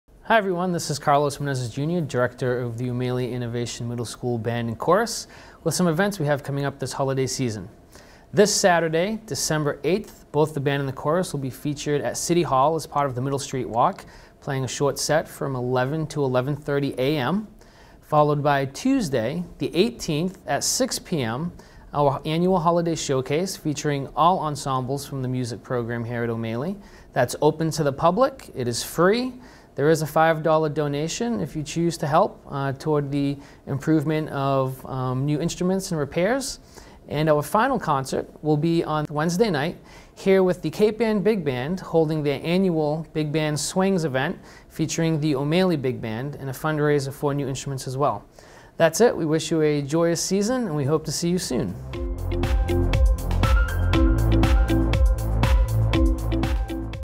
Need To Know: Education, Carlos Menezes, O'Maley Band and Chorus Dir.
Carlos Menezes Director of O'Maley Band and Chorus with an update on all the upcoming O'Maley Innovation Middle School holiday concerts.